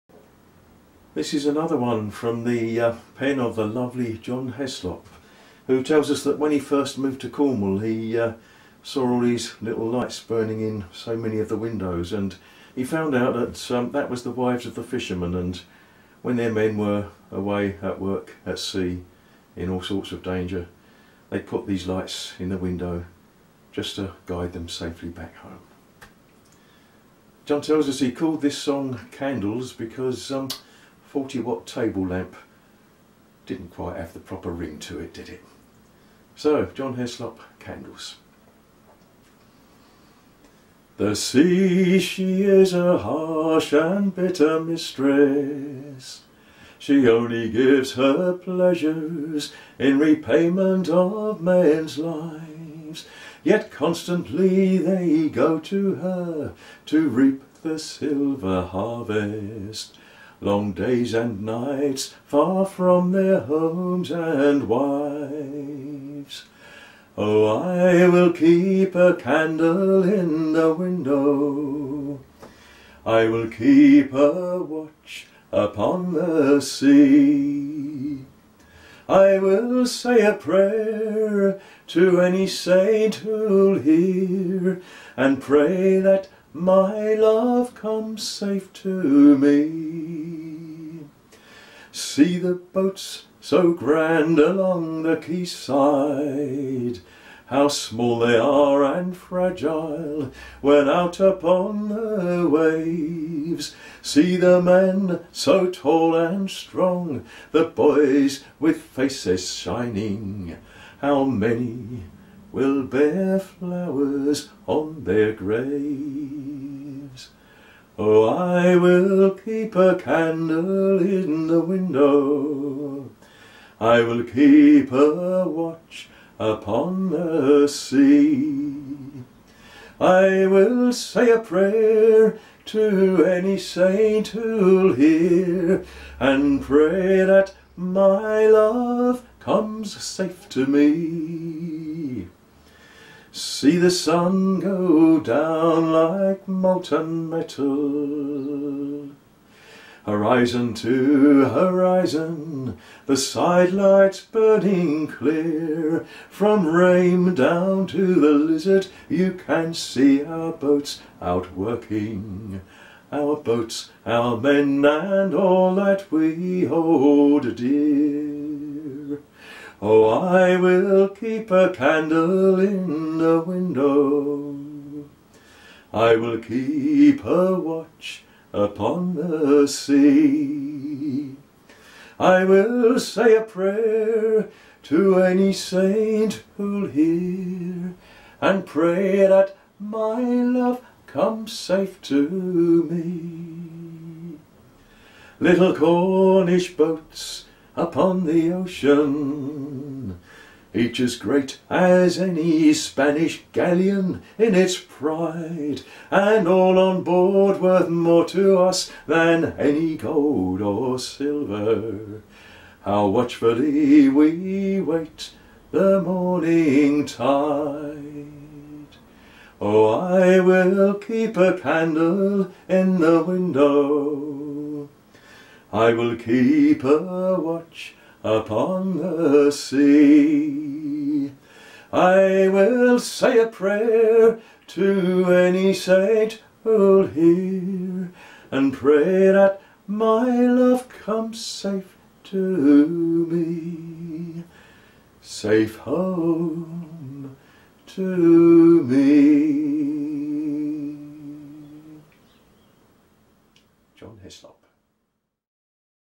candles
Jon Heslop's tale of the wives of Cornish fisherman guiding their men home from sea.